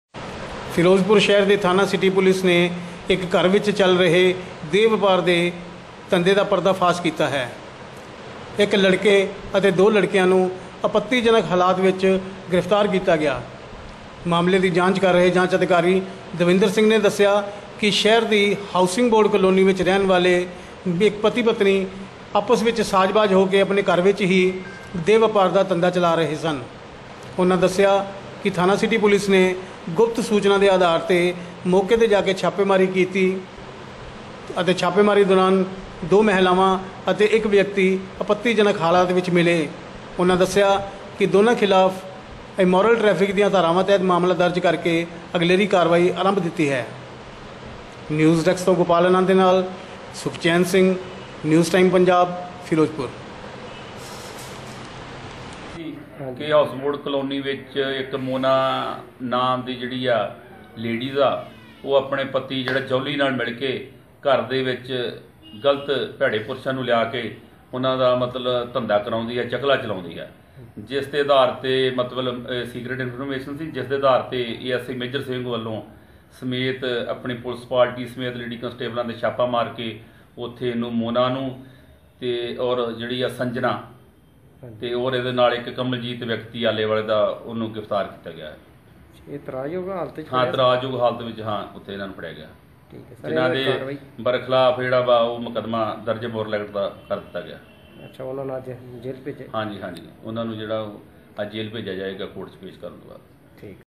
ਦੇਹ ਵਪਾਰ ਦਾ ਪਰਦਾਫਾਸ਼
ਦੋ ਔਰਤਾਂ ਸਮੇਤ ਤਿੰਨ ਕਾਬੂ